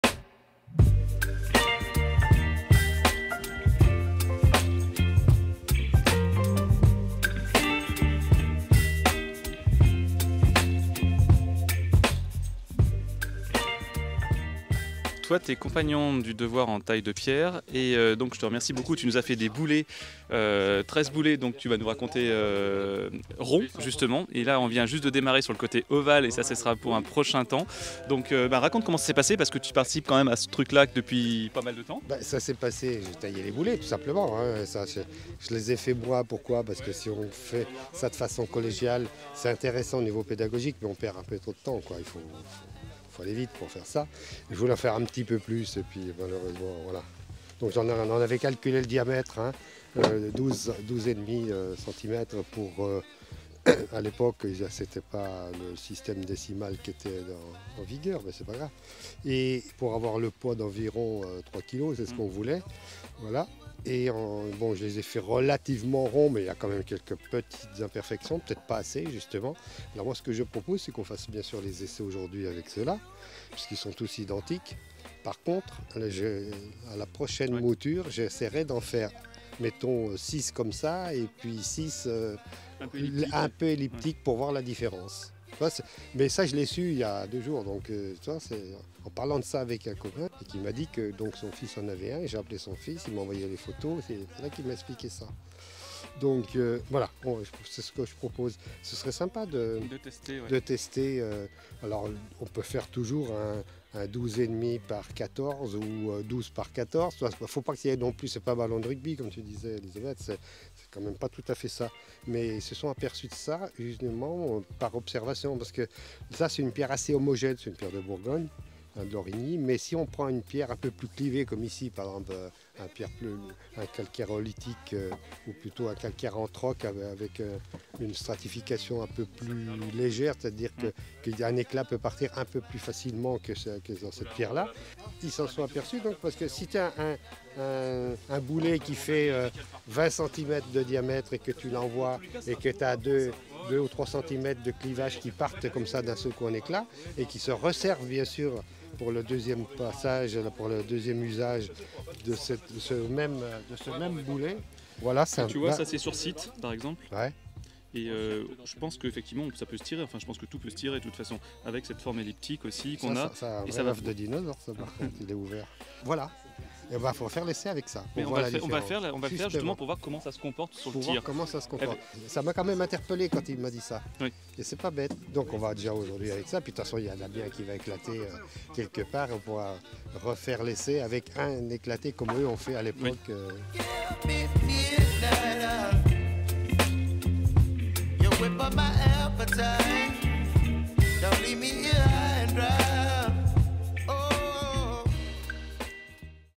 baliste2023 - Le compagnon tailleur de pierres - Jacques de Haese
👉 Jacques de Haese, compagnon du devoir tailleur de pierres, nous accompagne sur le projet d’archeologie expérimentale baliste2023 ⚒️

👉La baliste sera présentée auprès du Festival Européen Latin Grec au musee lugdunum le samedi 25 mars.